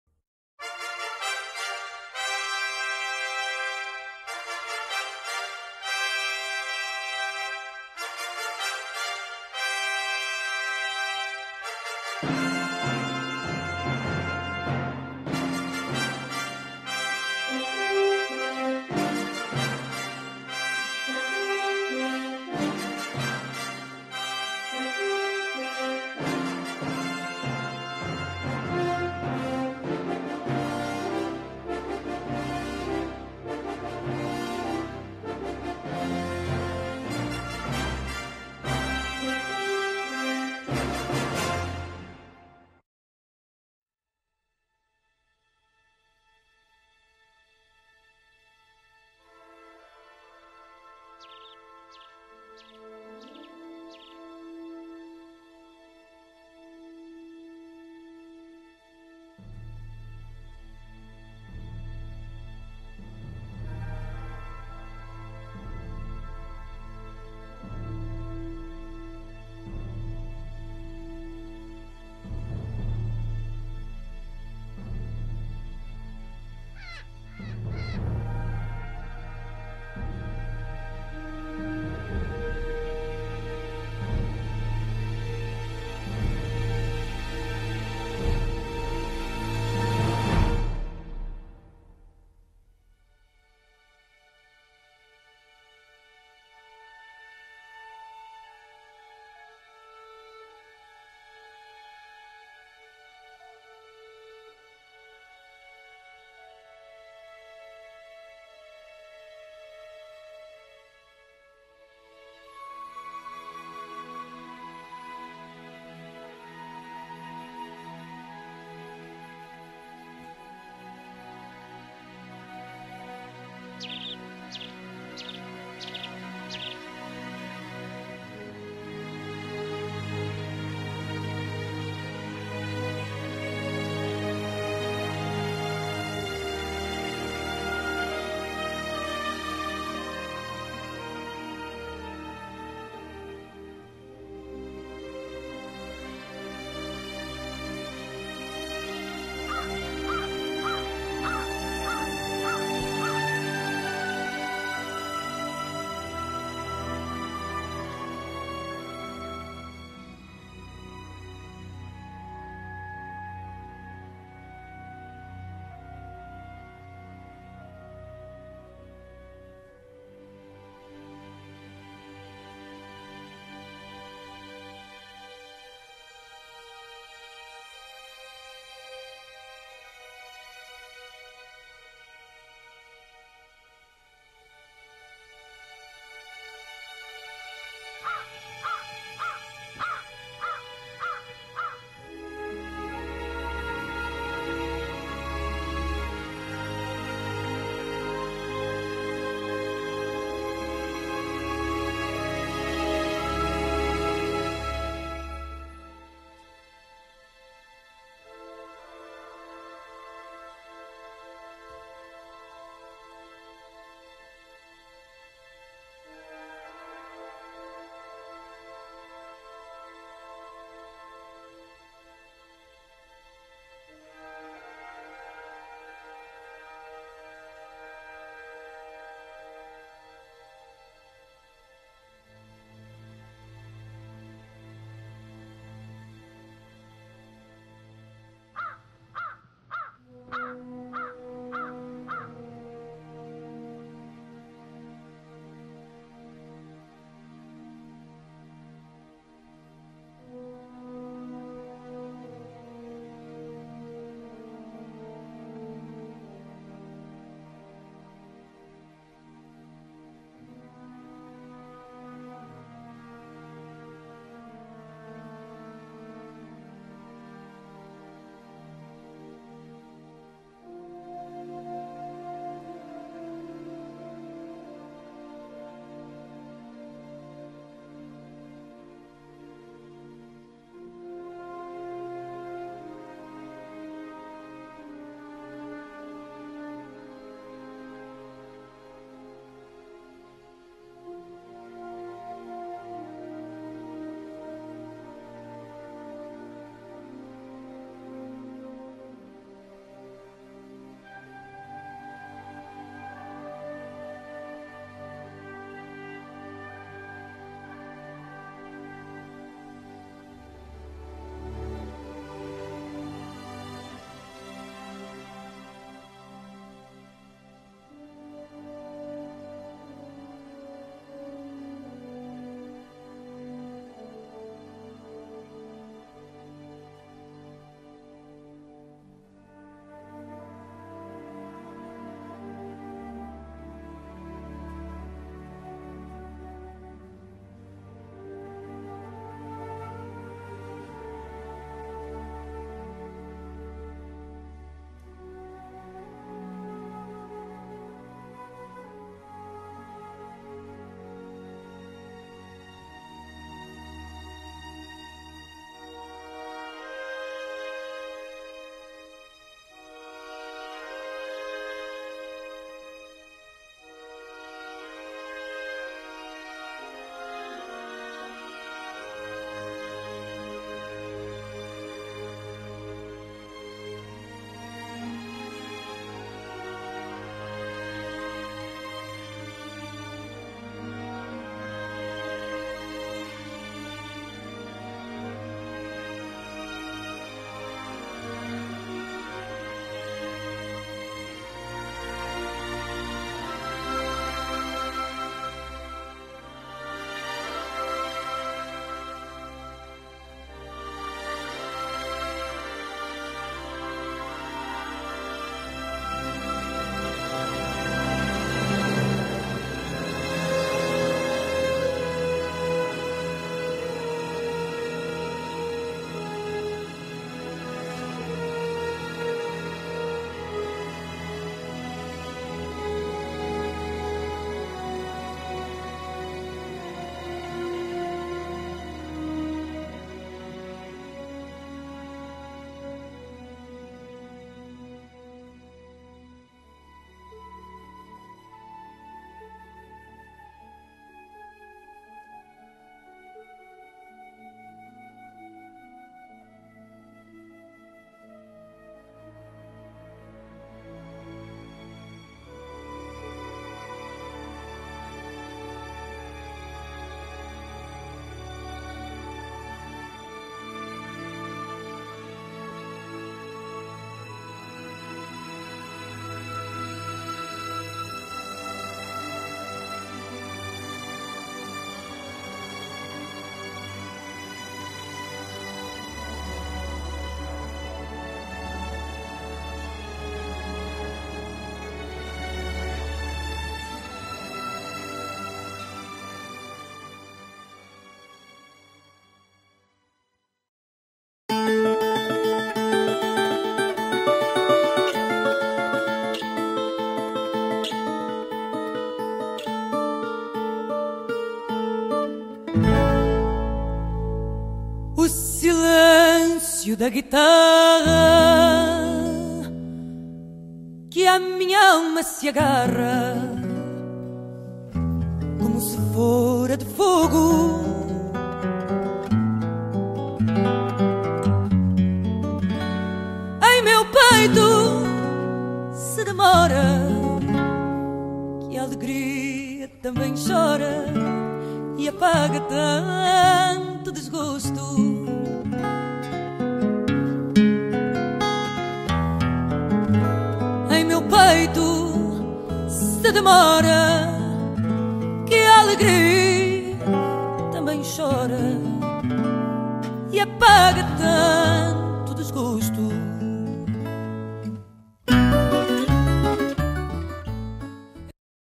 BRAGANÇA II CASTELO CASTILLO PORTUGAL
SEGUNDA PARTE DEL VIDEO DEL CASTILLO DE BRAGANÇA,CON SURECINTO AMURALLADO.